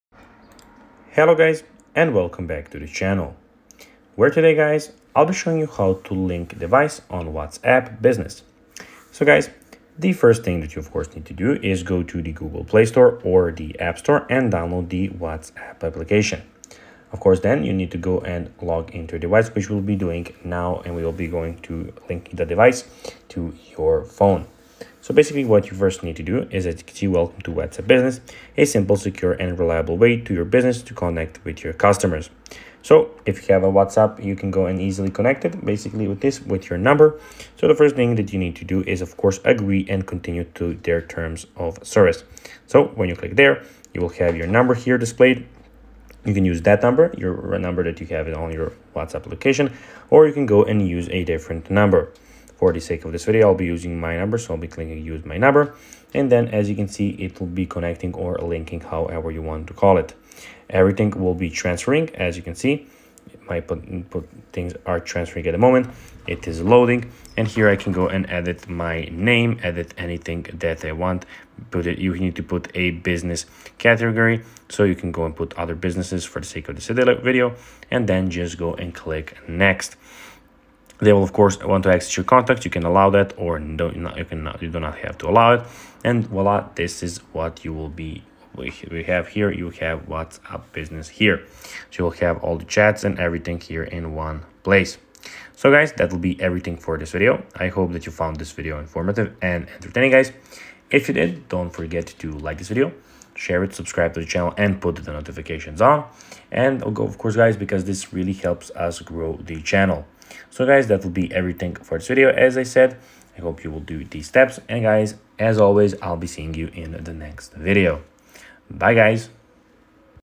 How To Link Device On WhatsApp Business (Quick Tutorial)
In this video I will show you How To Link Device On WhatsApp Business. It would be good if you watch the video until the end so that you don't miss important steps.